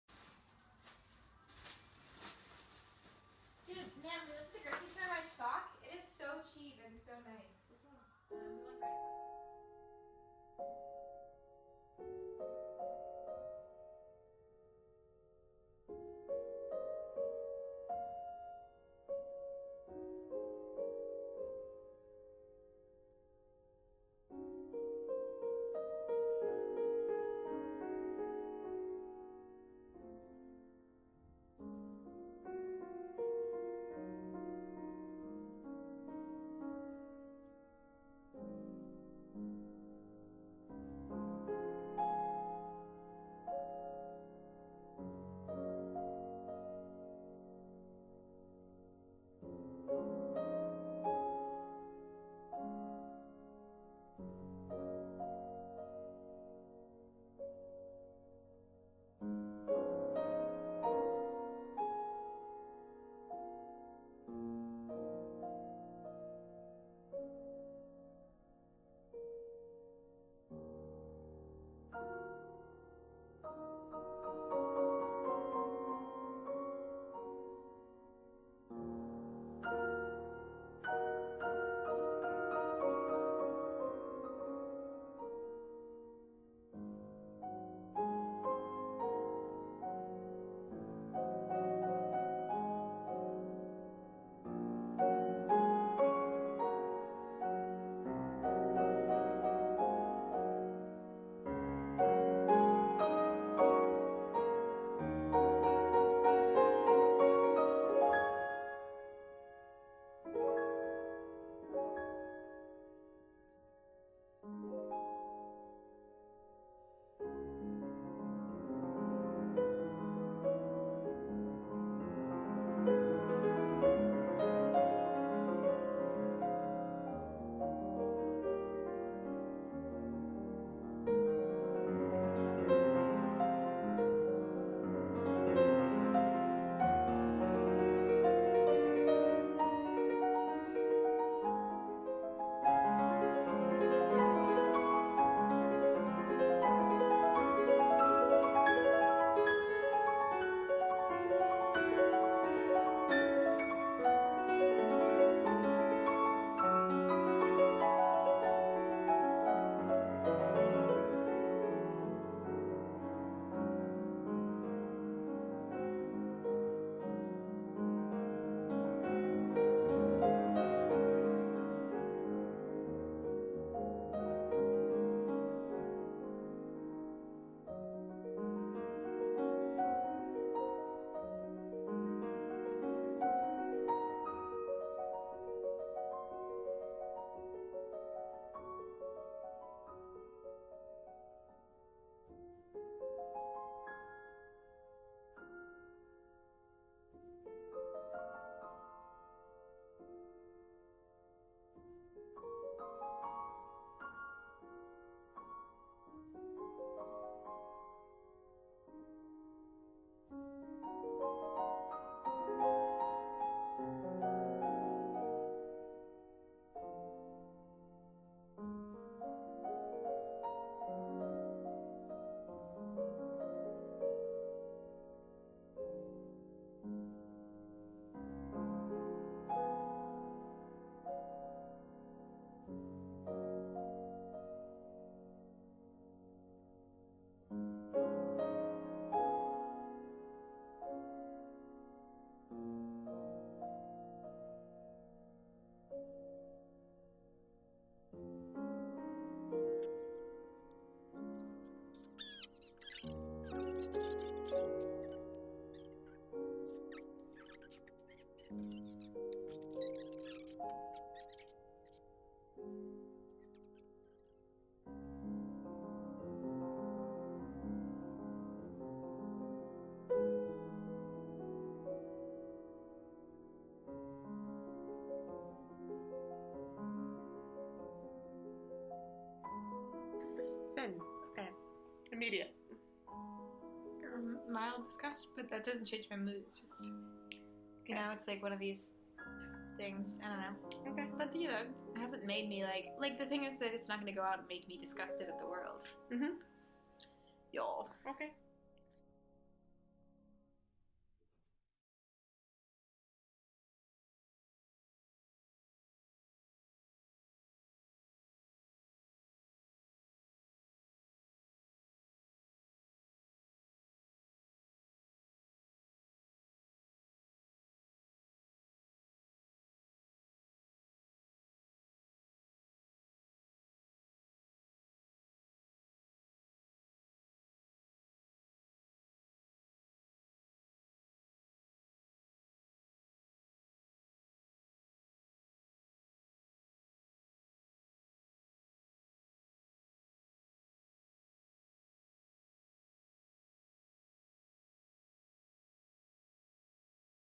Hidden Camera: Observing Emotional Contagion in My Flatmates
Culture Change & Exchange Seminar, Bard College Berlin Fall 2016